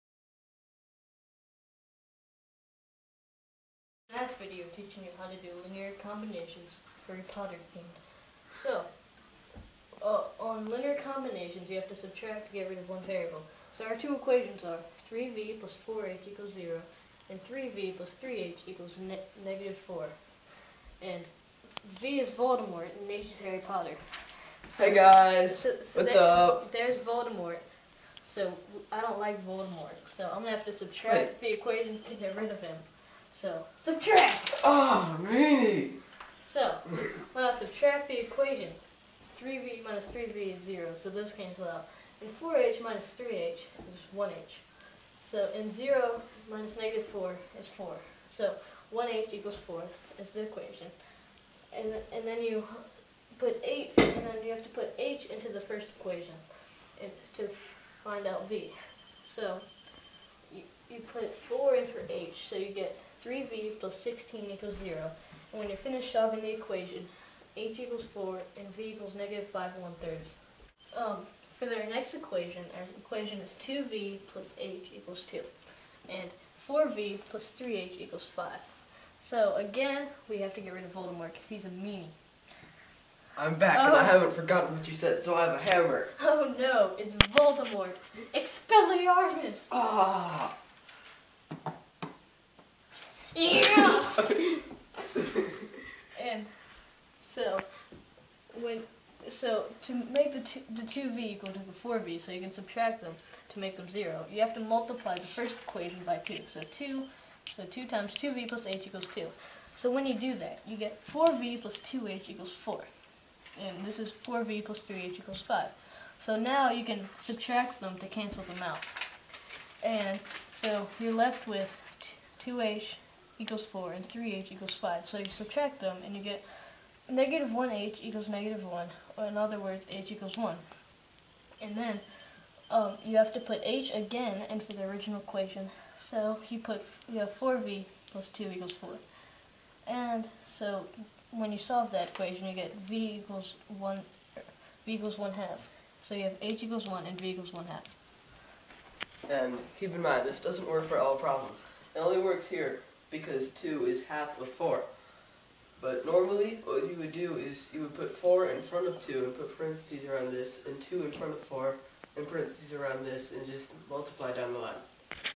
Linear Combination: The Harry Potter Way
Video for school about linear combination.